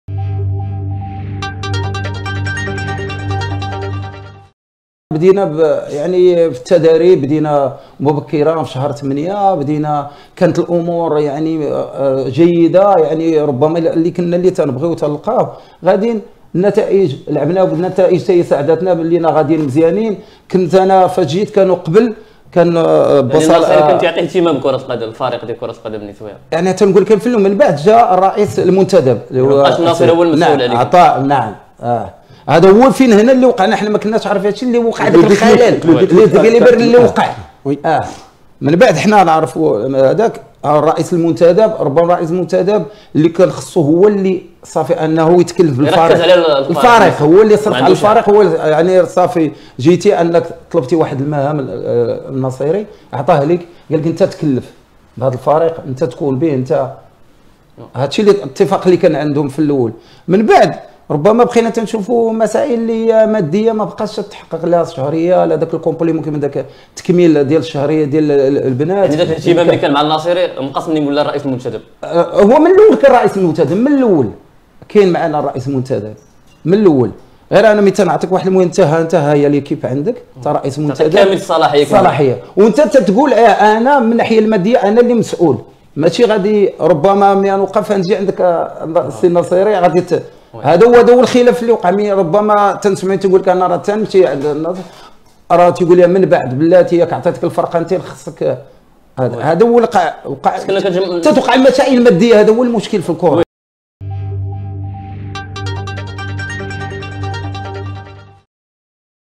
كشف الإطار الوطني عبدالله هيداموتجربته مع فريق الوداد النسوي في عهد الناصيري ورأيه في الرئيس المنتدب
عبدالله هيدامو يحكي تجربته مع فريق الوداد النسوي:في عهد الناصيري كنا غاديين مزيان منين جا الرئيس المنتدب لقينا مشكل فالماديات